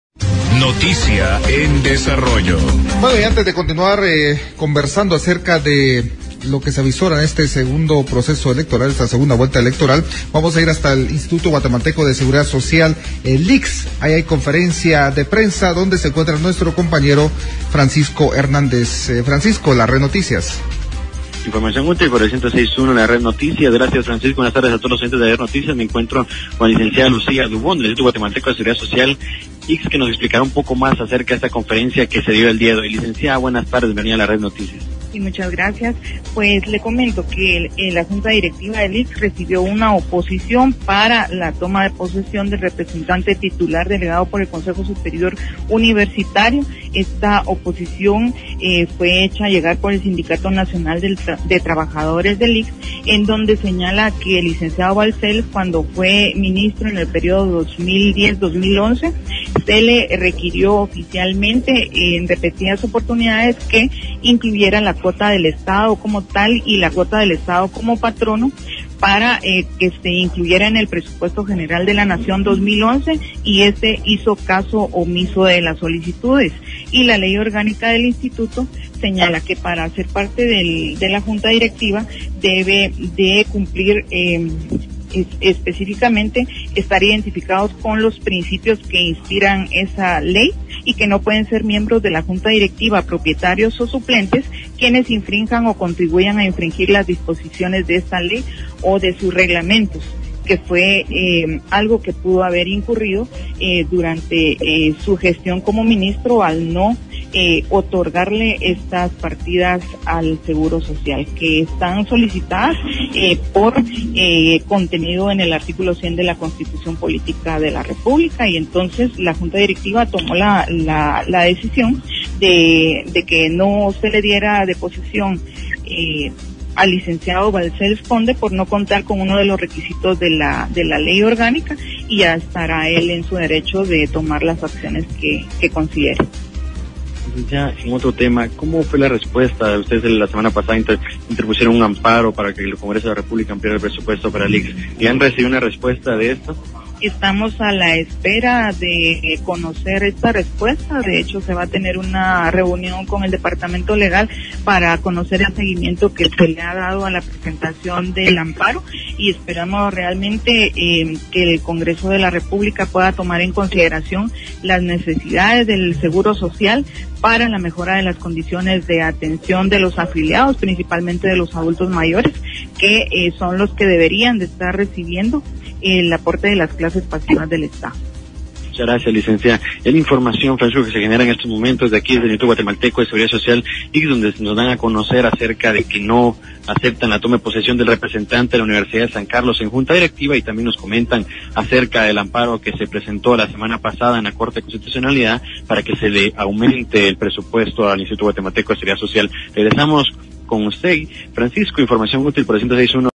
JD no dio posesión a Lic Balsells por oposición del Sindicato Nac. de Trabajadores
IGSS informó que JD no dio posesión a Lic Balsells por oposición presentada por Sindicato Nac. de Trabajadores